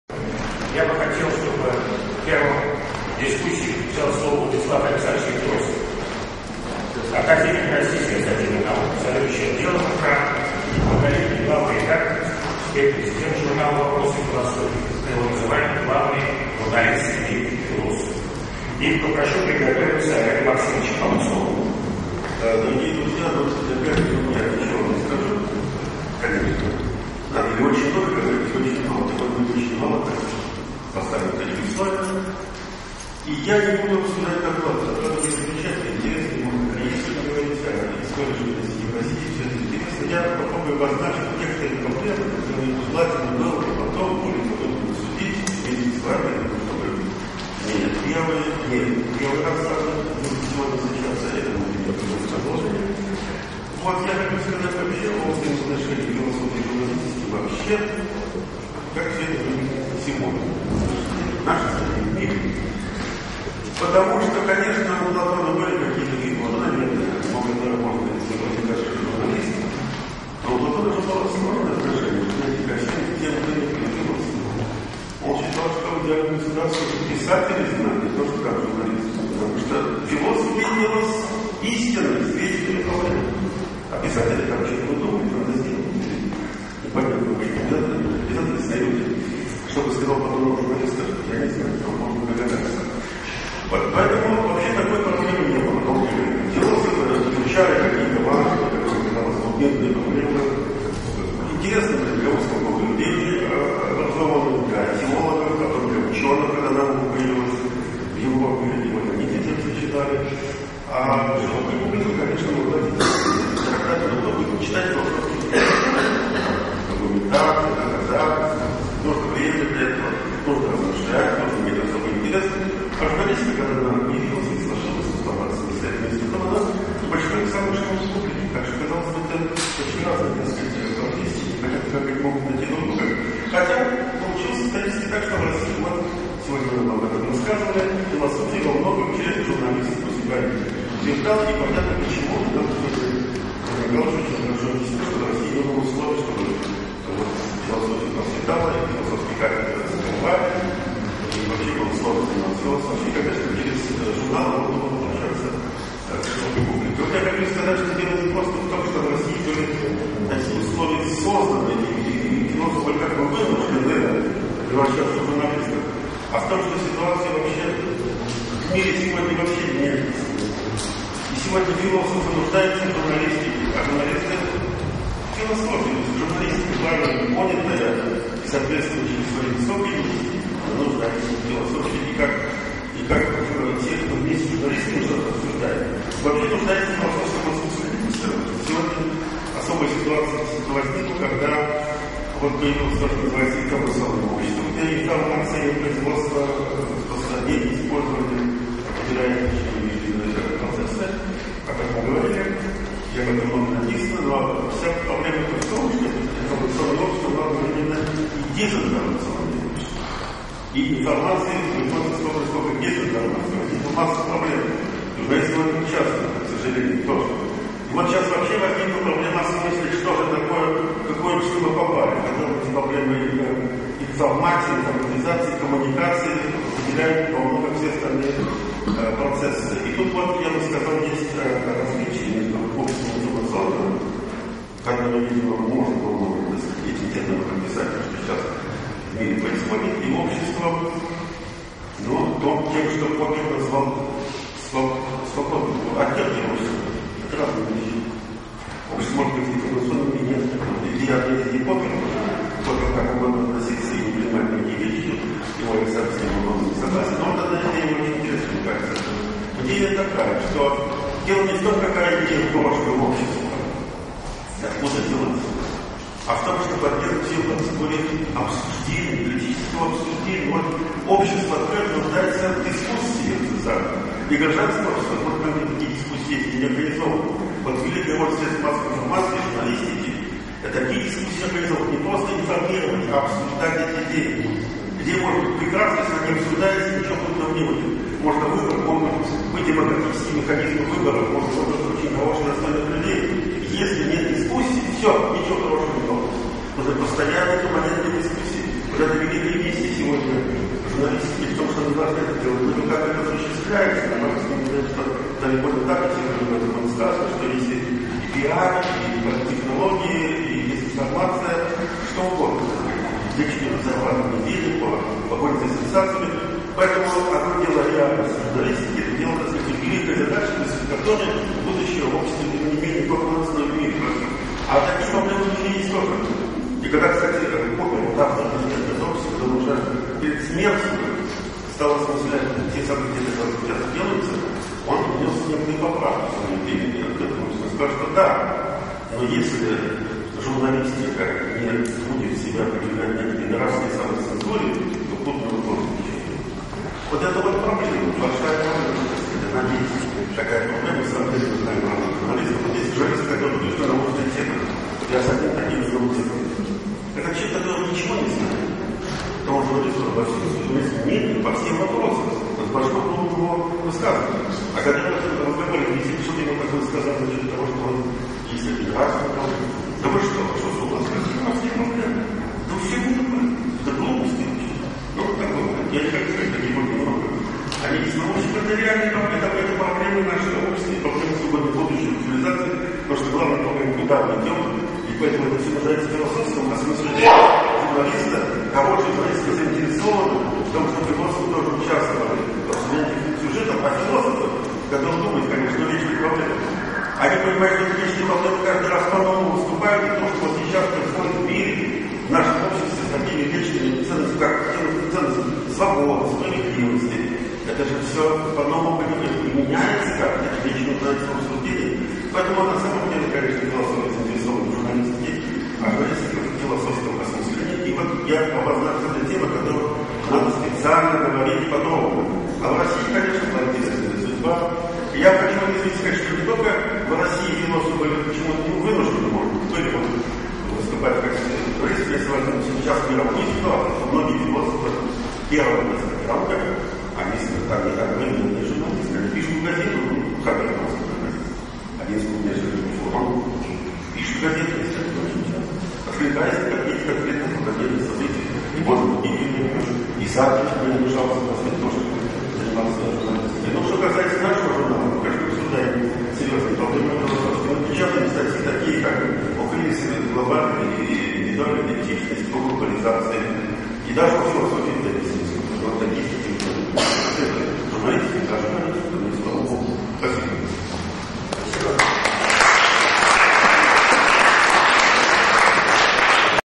Владислав Александрович Лекторский
Симпозиум «Философия и журналистика», Институт философии РАН , 11 мая 2010 года. В ходе дискуссии выступил академик РАН, зав. отделом ИФ РАН, президент журнала «Вопросы философии» Владислав Александрович Лекторский